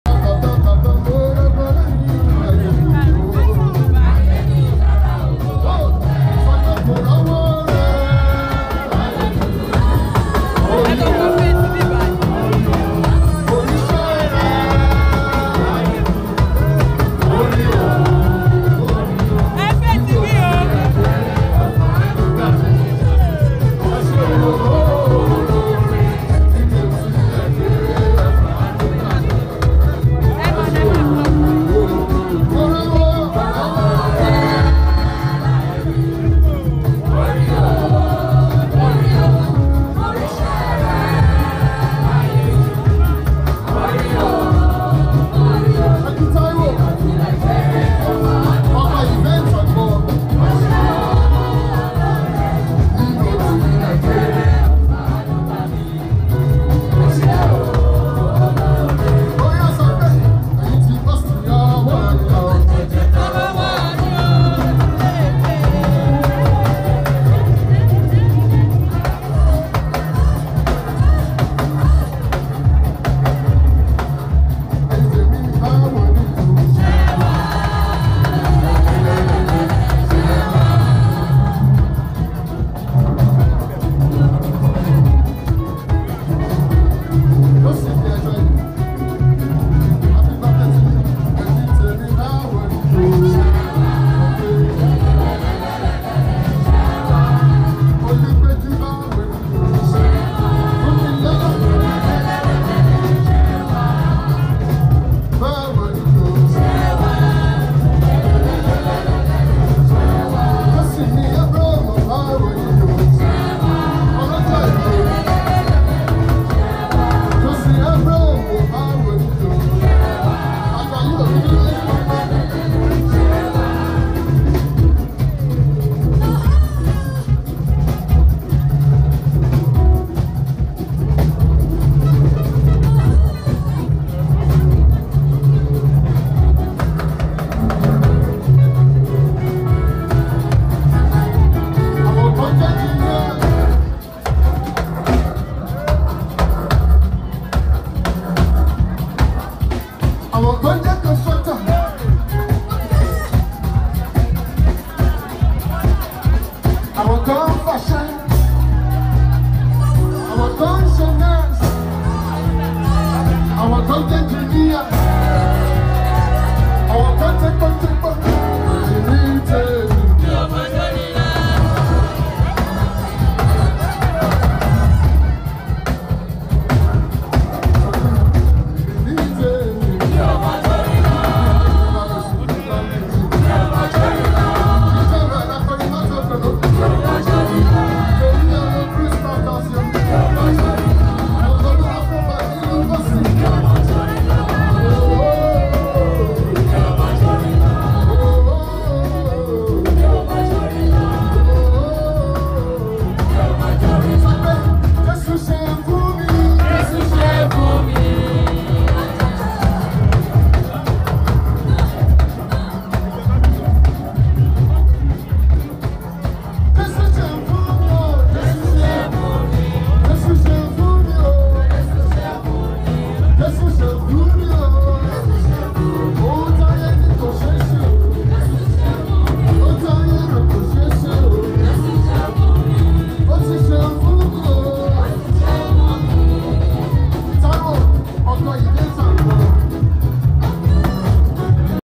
ABRAHAM AJAYI'S 50TH BIRTHDAY PARTY RG_GIST EVENTSANDPARTIES CAPTURE THE LIT HIGHLIGHTS
RG_GIST EVENTSANDPARTIES CAPTURE THE LIT HIGHLIGHTS